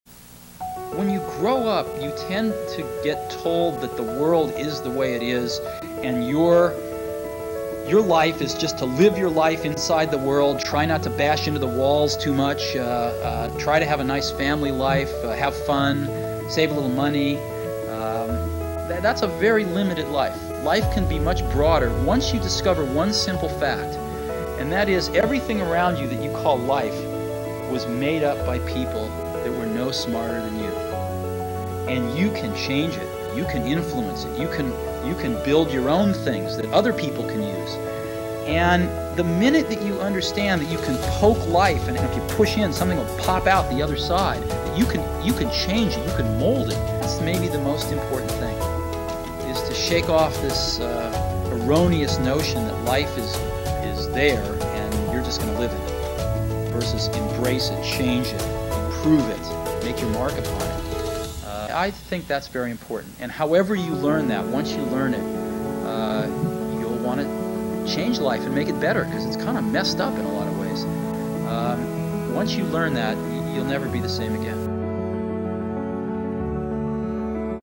Best advise from Steve Jobs
I recently heard this and I had to share it. The world is just as someone imagined it and shaped it. We have to power to change our circumstances and the world around us. Never stop dreaming and never stop chasing your dreams.